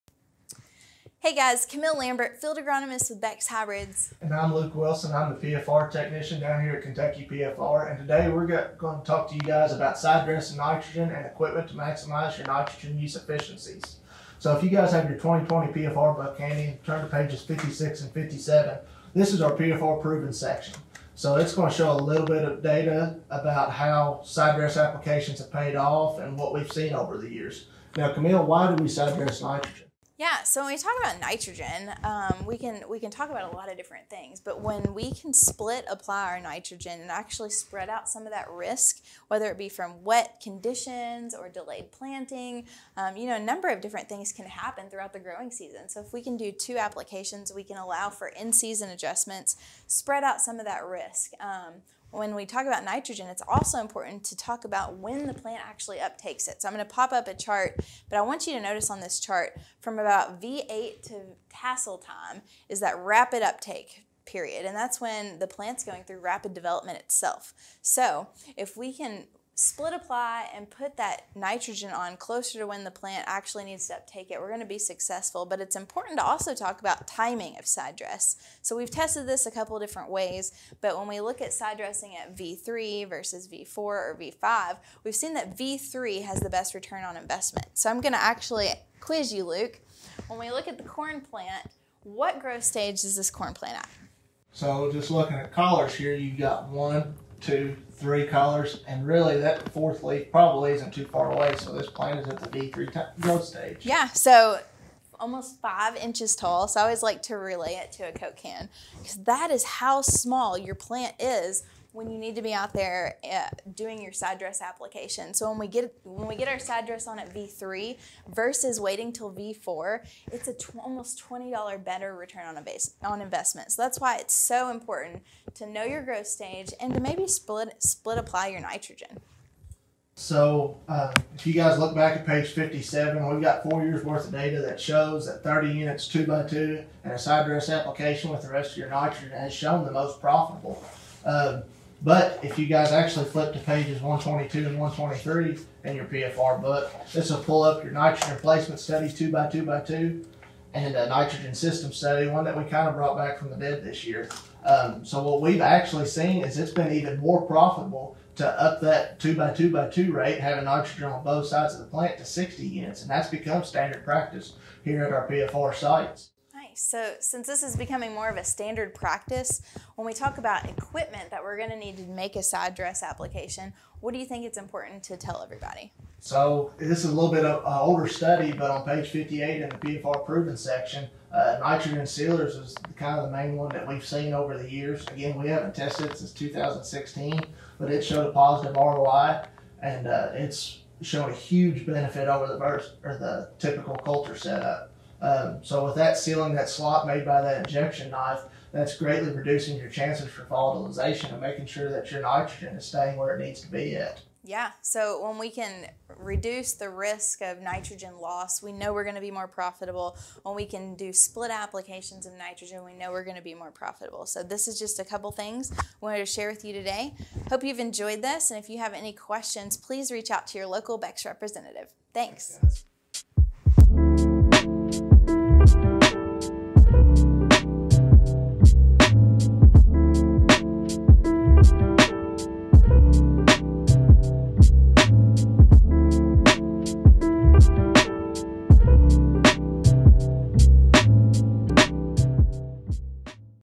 Maximizing Nitrogen Use Efficiency - PFR Book Findings | Beck's Agronomy Update
Nitrogen is one of the three primary macronutrients a corn plant needs for essential growth and development. Discover in this video which factors of profitability to consider when split applying nitrogen on corn. Camille Lambert, Field Sales Agronomist, and Luke Wilson, Kentucky PFR Technician discuss Practical Farm Research (PFR®) data on which corn growth stages to target for split-applied nitrogen applications, what equipment is most efficient, and the science behind corn nitrogen uptake.